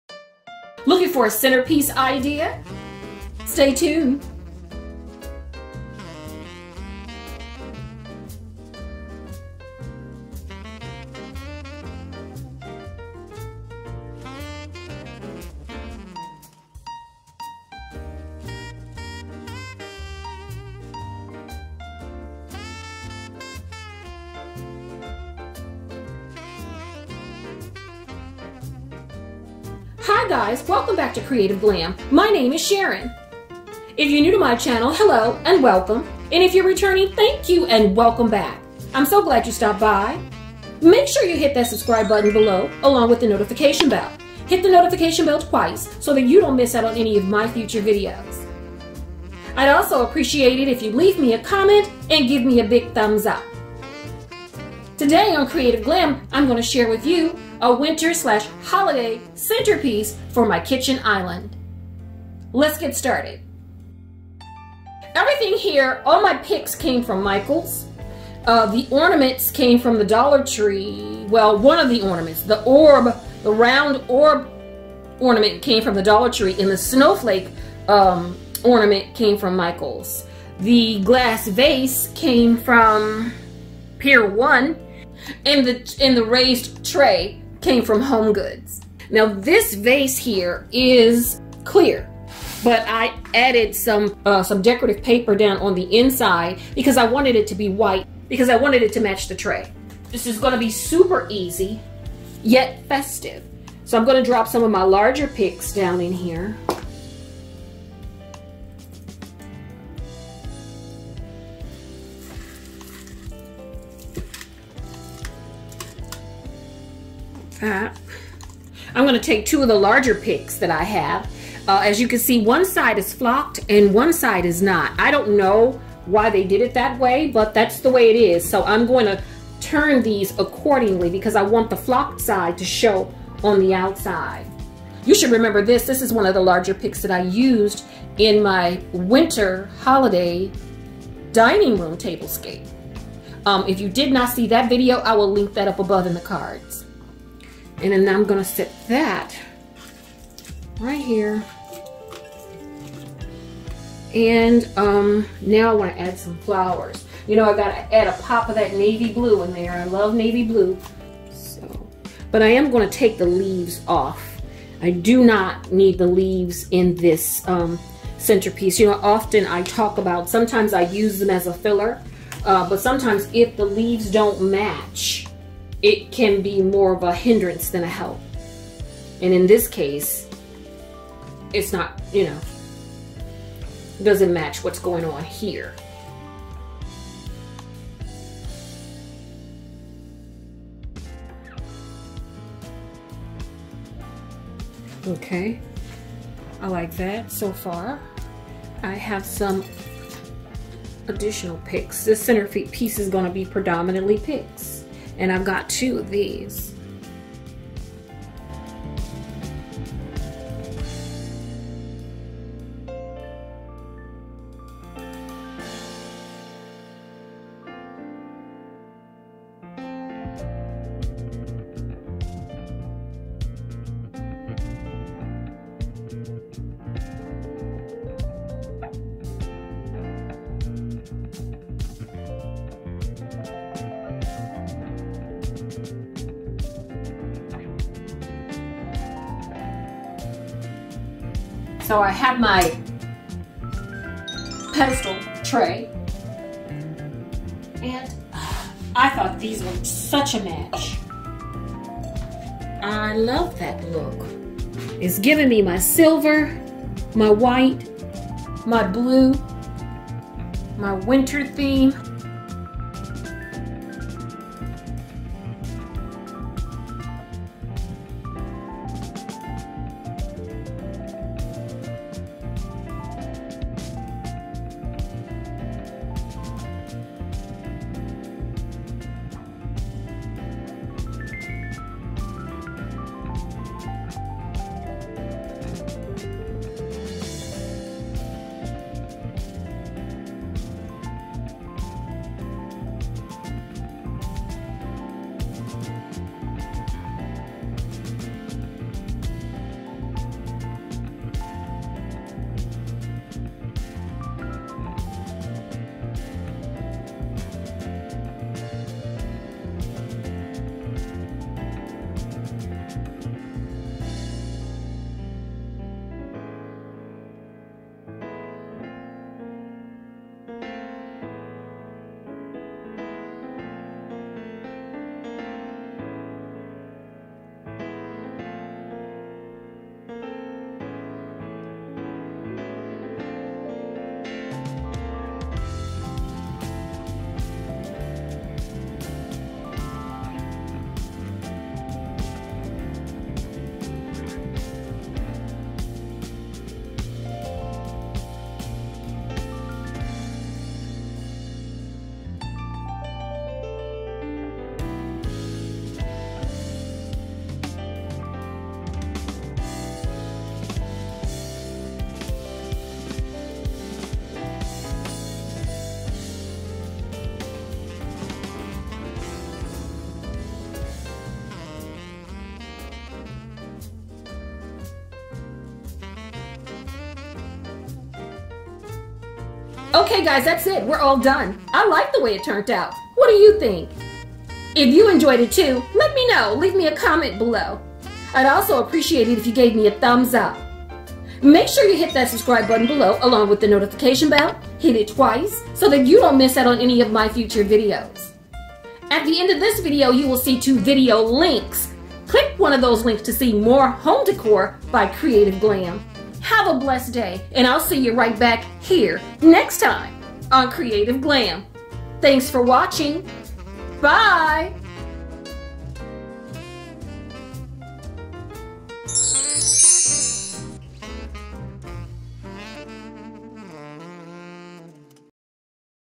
Quick & Easy Christmas Decor Idea || Kitchen Island Centerpiece
Hi Guys! 💖 Today I will share my Quick & Easy Christmas Decor Idea || Kitchen Island Centerpiece. I hope you enjoy. I love What I Do and I Do What I Love. 💖 christmasdecor ideas

📷 Lighting Kit